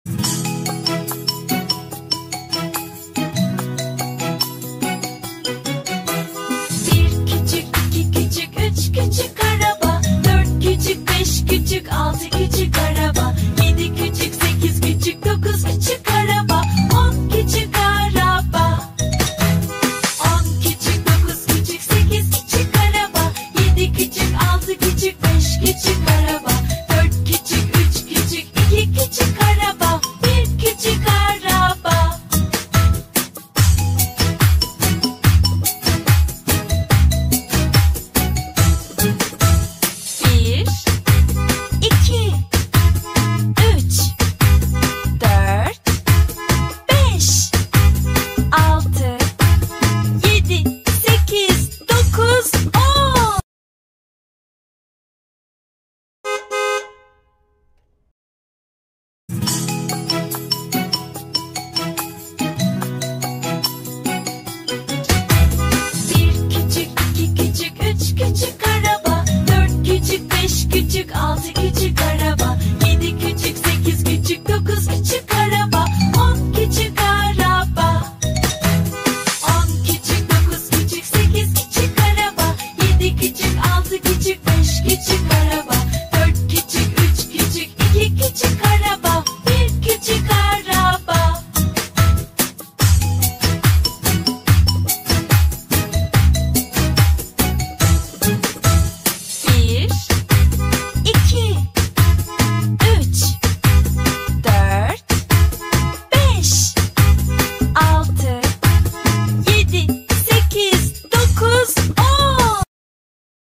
10 Küçük Araba -Sayı Saymayı Öğreniyorum
Merhaba çocuklar! 10 küçük araba ile sayı saymayı öğrenmeye hazır mısınız? Bu eğlenceli çizgi filmde, renkli karakterlerimizle birlikte sayılar dünyasına adım atıyoruz. 1'den 10'a kadar olan sayıları keşfediyor, nasıl yazıldıklarını öğreniyor ve onları eğlenceli oyunlarla pekiştiriyoruz.

Her bir küçük arabayı tanıyacak, onların yardımıyla sayıları sayacak ve sayıları doğru sırayla yerleştireceğiz. Şarkılarımız eşliğinde dans edecek, boyama etkinlikleriyle sayıları daha iyi kavrayacak ve matematik becerilerimizi geliştireceğiz.

Bu eğitici çizgi filmde, sayıları öğrenmek hiç bu kadar eğlenceli olmamıştı! Hazır mısınız? 10 küçük araba ile birlikte sayı saymayı öğrenmek için şimdi izlemeye başlayalım ve matematik dünyasına adım atalım!